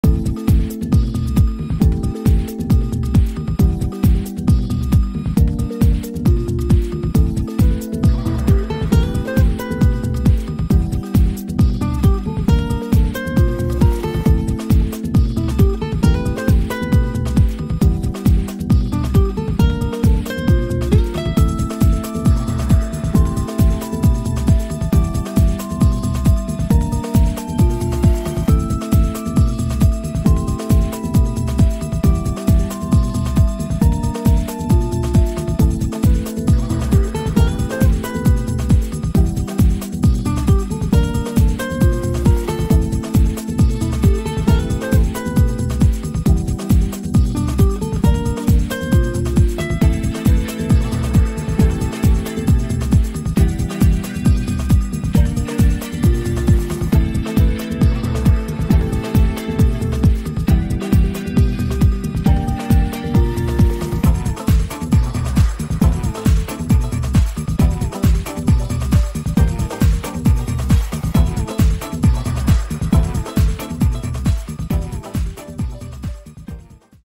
Ace Physiotherapy Downtown Toronto ON 360 Tour
Ace Physio for Expert Physiotherapy in Downtown Toronto at the corner of Yonge St and College St.  TTC Accessible.